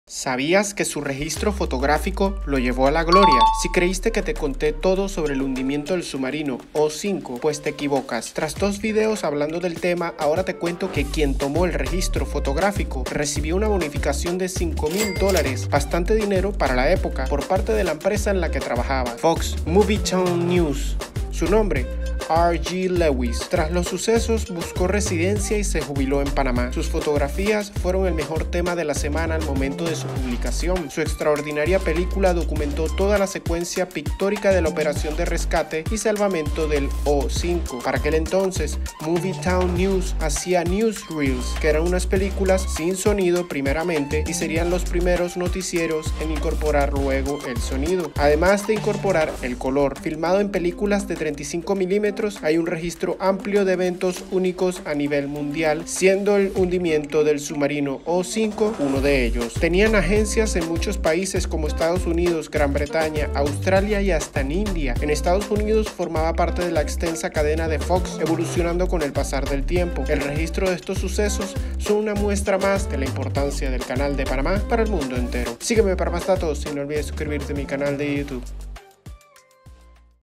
Un registro para toda la vida (continuación del hundimiento del submarino O-5)
Y si creíste que te conté todo sobre el hundimiento del submarino O-5, pues te equivocas. Tras dos videos hablando del tema, ahora te cuento que quien tomó el registro fotográfico recibió una bonificación de cinco mil dólares, bastante dinero para la época.

And if you thought that I told you everything about the sinking of the O-5 submarine, then you are wrong. After two videos talking about the subject, now I tell you that whoever took the photographic record received a bonus of five thousand dollars, quite a lot of money for the time.

Videos & fotos & edición: @matos0893 (Andrés Matos) & Internet & Pexels & YouTube.